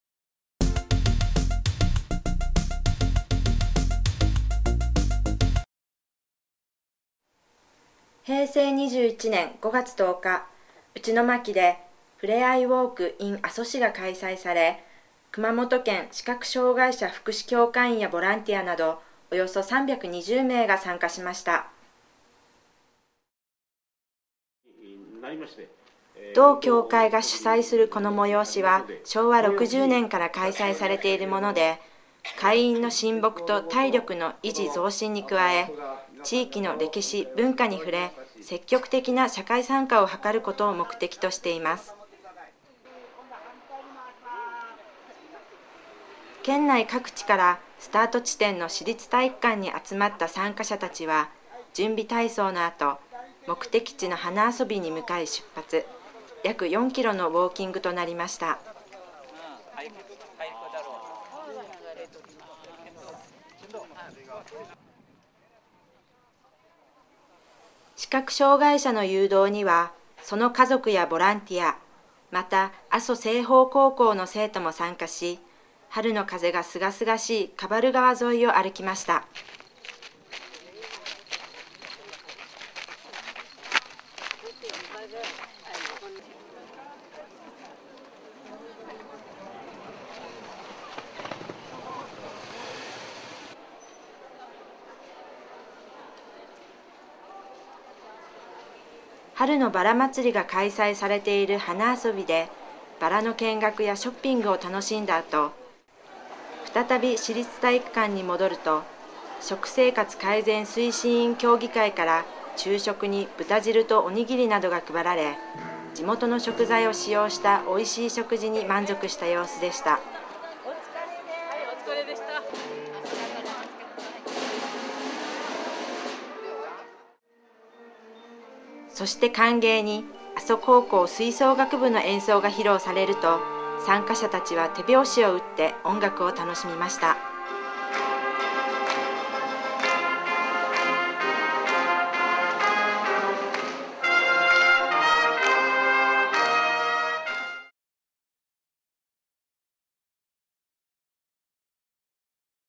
平成21年“ふれEYEウォーク”が阿蘇市で開催
県視障協会が主催する“ふれEYEウォーク”が阿蘇市で開催されました。